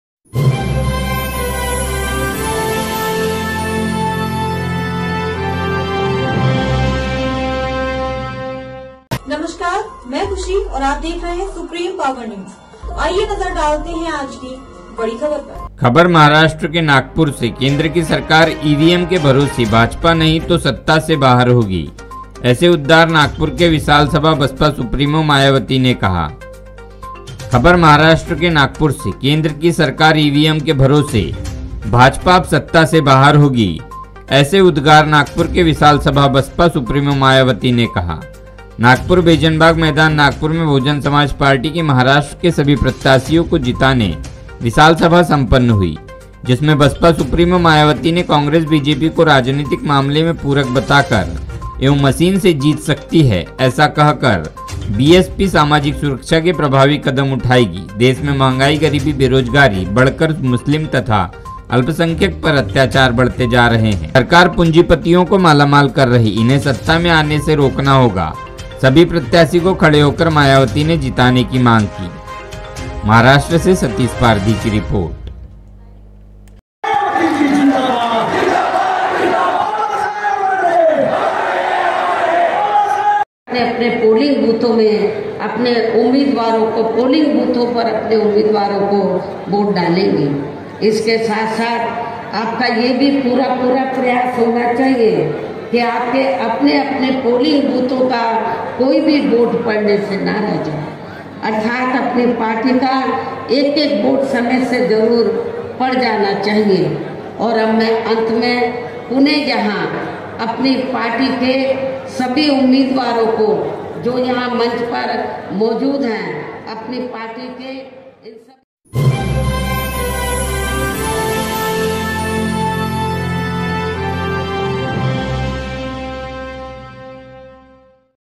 केंद्र की सरकार इवीएम के भरोसे भाजपा अब सत्ता से बाहर होगी -बसपा सुप्रीमो मायावती
नमस्कार दोस्तों,